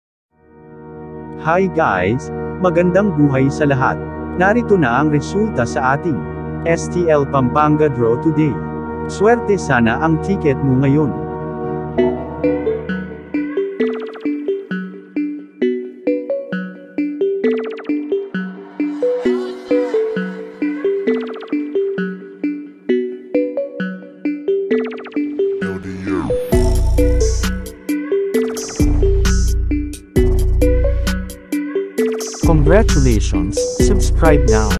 1ST DRAW, STL PAMPANGA 11AM Result Today November 24, 2023 Morning Draw Result Philippines
Today November 24, 2023 Friday
STL Small Town Lottery, First draw result for STL PAMPANGA STL Pares Game.
STL PAMPANGA 1ST DRAW RESULT TODAY
GAME: STL PAMPANGA 1ST DRAW
STL PAMPANGA live draw today 1st draw

Here is the STL PAMPANGA Result Today (11:00 AM DRAW RESULT).